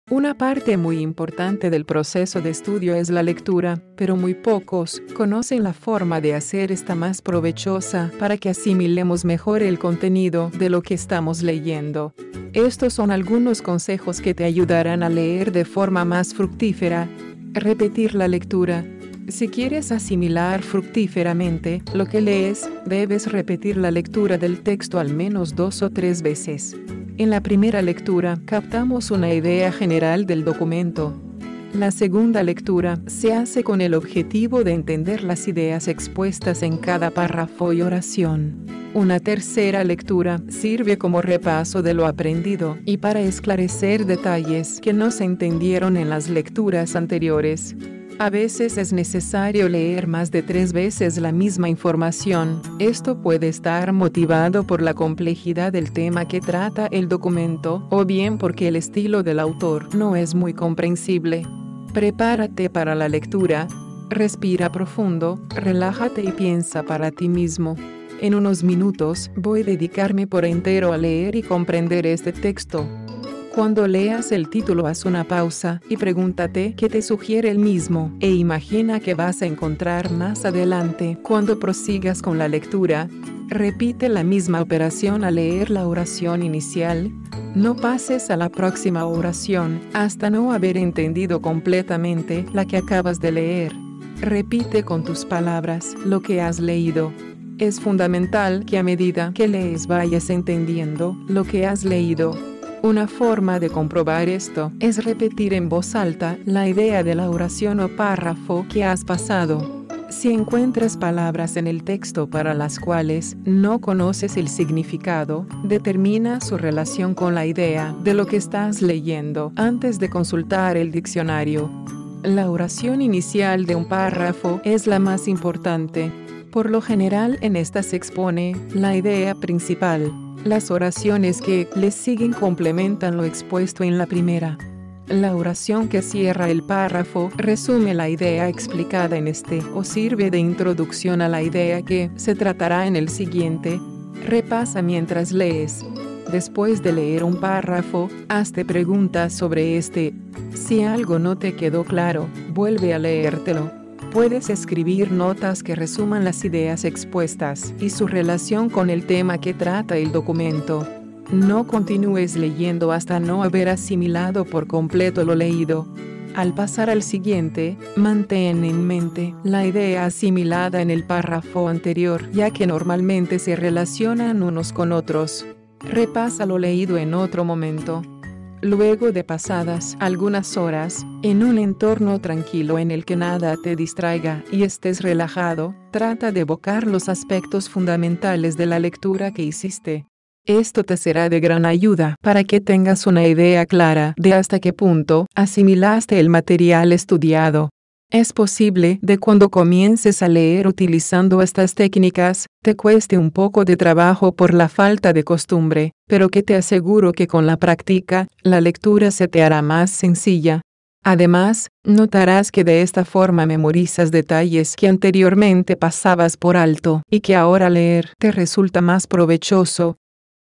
Como leer bien
Cómo se puede leer bien y comprender mejor la lectura. Mira el artículo en la web